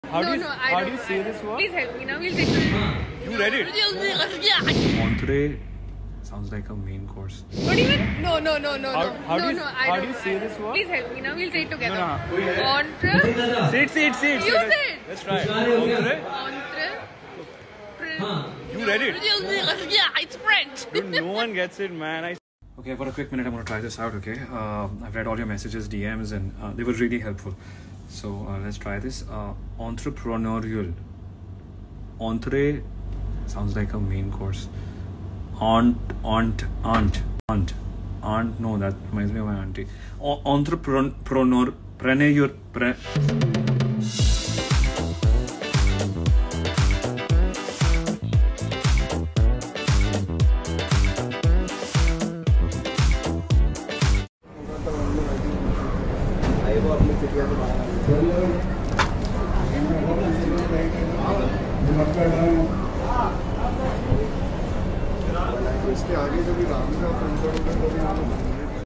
Nakuul Makes FUN OF Disha😂😂. Teaches Her English Vocabulary | Fun BTS Video
Here we bring you fun BTS videos Of Nakuul Mehta teaching co-star Disha Parmar English. The fun banter videos are going viral. Watch the video to know more.